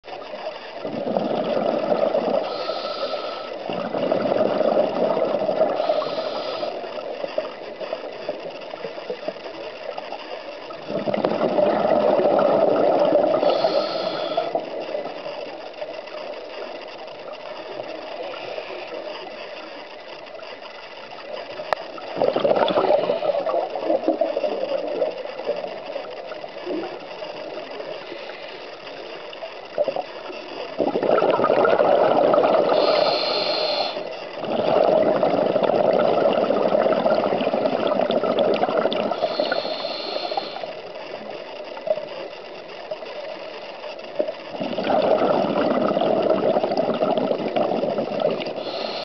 Cuttlefish intelligent invertebrates
Atlantic cuttlefish in the north of Portugal, filmed 14 February 2010
Mollusca Cephalopoda Sepiidae - Choco - Cuttlefish - Seiche